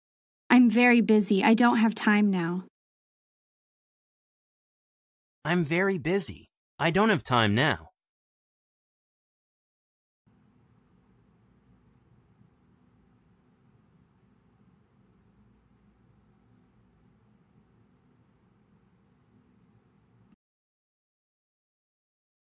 How to say "I'm very busy. I don't have time now."
Male Voice:
I'm very busy. I don't have time now.